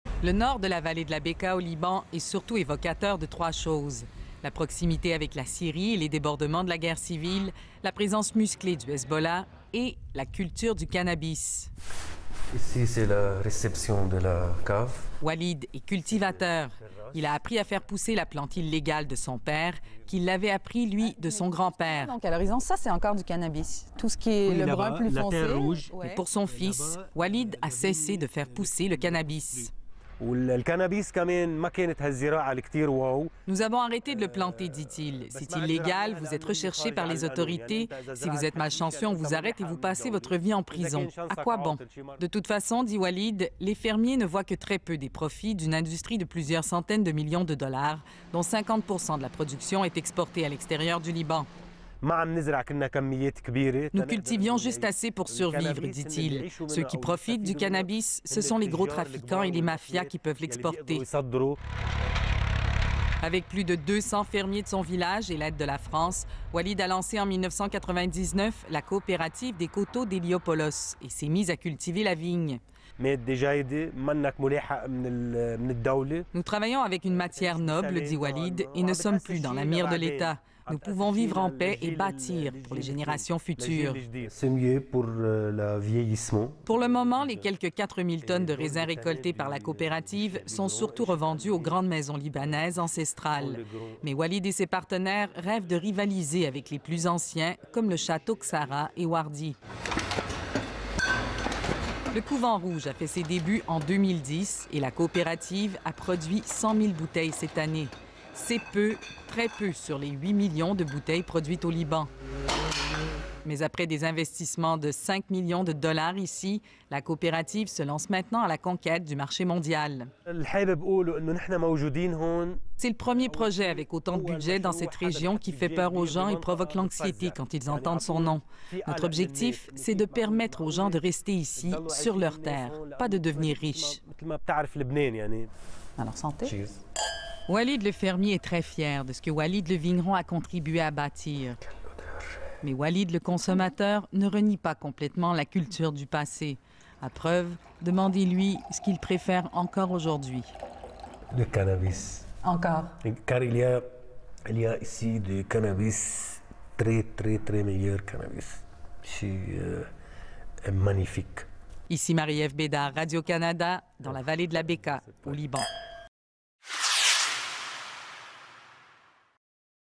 Du cannabis... à la viticulture!
Alors que la marijuana sera bientôt légalisée au Canada, la culture de cette plante demeure passible de prison ailleurs dans le monde. C’est le cas notamment au Liban, qui demeure malgré tout l'un des cinq plus gros producteurs mondiaux de cannabis. Mais certains cultivateurs en ont assez d'être dans la mire de la police. Notre correspondante Marie-Eve Bédard en a rencontré un qui a décidé de se convertir à la viticulture.